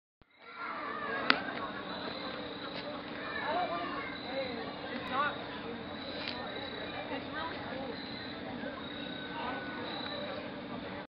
Jack O Lantern
David's Jack O Lantern to attract people to the yard...and keep everyone warm.